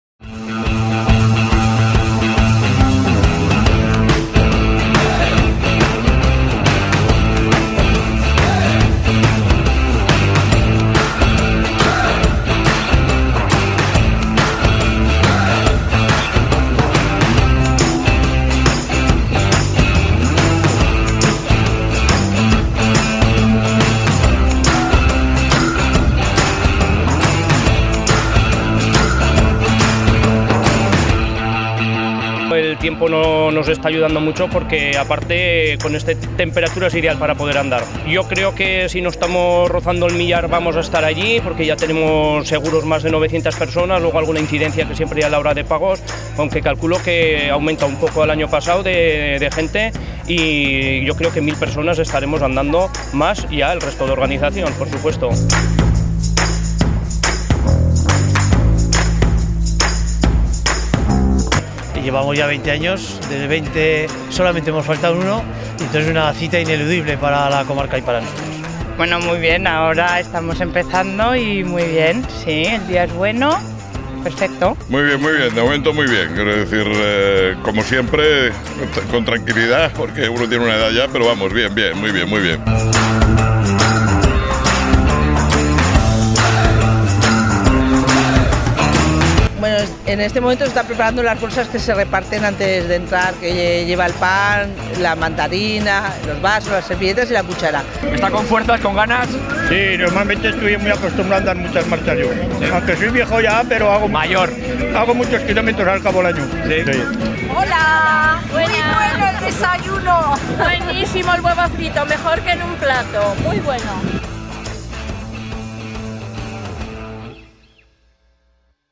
La Comarca.tv - XX Marcha Senderista del Mezquín
La marcha senderista del Mezquín cumplió su edición número veinte y más de mil senderistas partieron desde Castelserás para recorrer tres tramos diferenciados por el kilometraje. Como siempre los voluntarios fueron pieza clave en la organización y como novedad en el puesto de avituallamiento número tres se dieron huevos fritos.